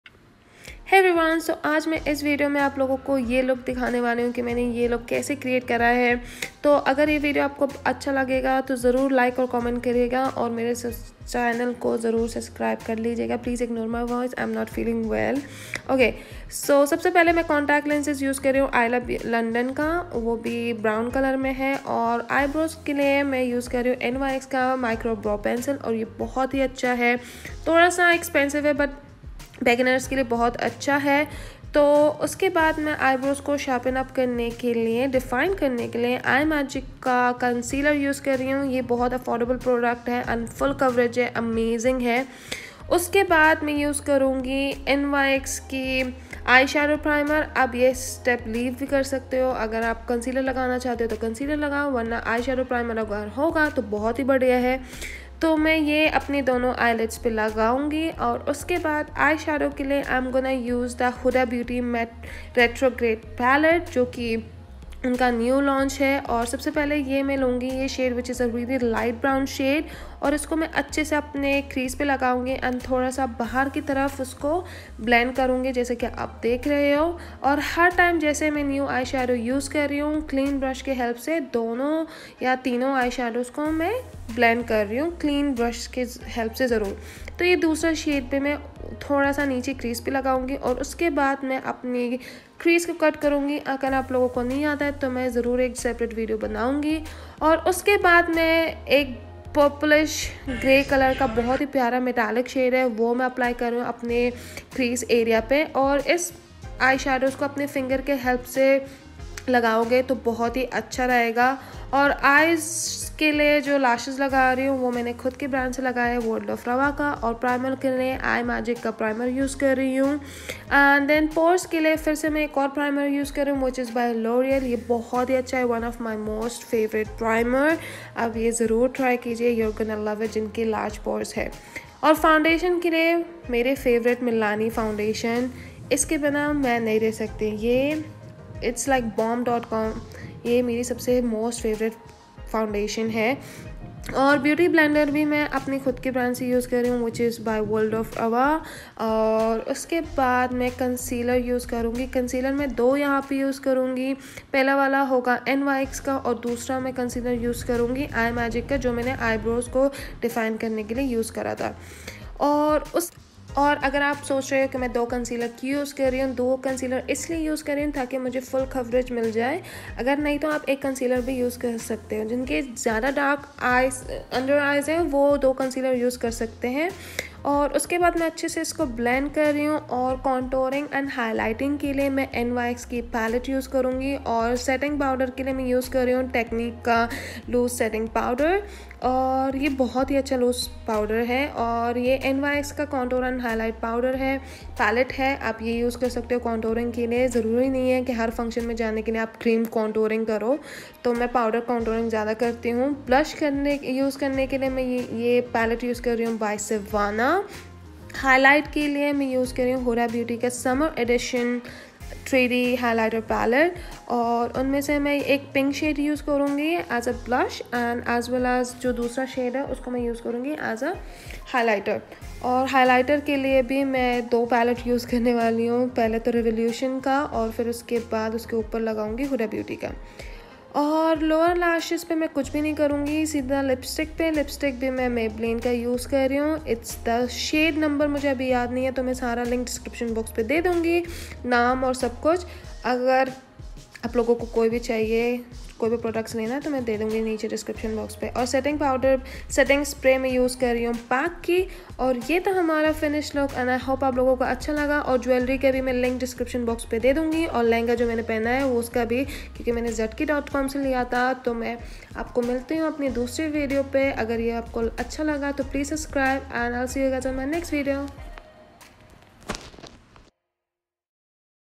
Grwm For My Friends Wedding | Had To Hurry Because Everyone Was Waiting 😱| hudabeauty retrograde
Get ready with for my friends wedding , just created this look in such a hurry I hope you guys will like it .

I’m using all of my favourite products in this video .

Hudabeauty mercury retrograde palette is just amazing I’m totally in love with it .

Products used:- Face :- @imagiccosmetics  pore filler primer .

@lorealmakeup base magique primer .

@milanicosmetics 2 in 1 foundation in shade creamy vanilla.

@nyxcosmetics_in HD concealer shade fair .

@imagiccosmetics concealer.

@nyxcosmetics_in contour and highlight palette.

@hudabeauty summer edition 3D highlighter palette.

@makeuprevolution Sophie highlighter palette.

@paccosmetic micro setting spray Eyes :- @nyxcosmetics_in micro brow pencil.

@worldofrawa ICEY wifey lashes .

@nyxcosmetics_in epic black liner .

@maybelline lipstick in shade lover. Jewellery by @tnc_wholesale_and_retail Dress by @zatkionline
 Contact lenses :- ayla London brown